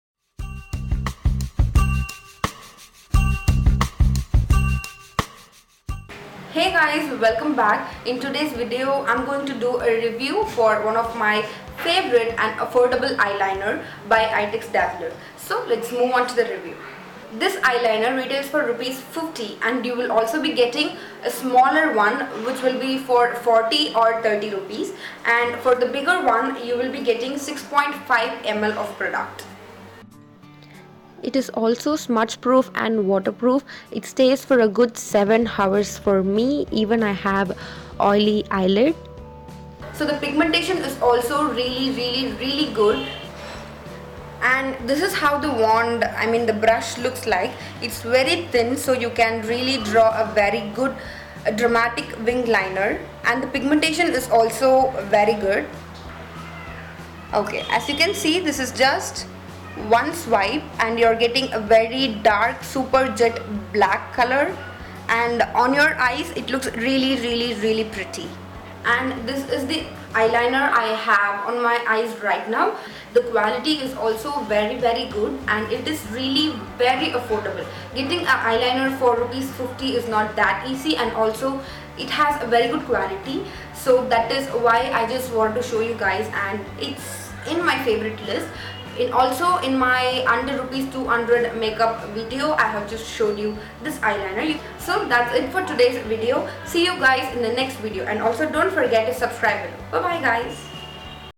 Best eyeliner under rs.50 to draw winged liner | Eyetex dazller | Holygrail1 | Keerthi shrathah
Hello homies :) Happy day... i have started a new series called Holygrail in this series i will be reviewing all the products i love so much and you can enjoy watching my favourite products so far.So the first products in the list os the eyeliner by eyetex dazller.Hope you like it.

you can buy this eyeliner in your local store.

Eyetex dazller eyeliner - Rs.50

Simple Hairstyle videos for beginners

You can be friends with me ❤❤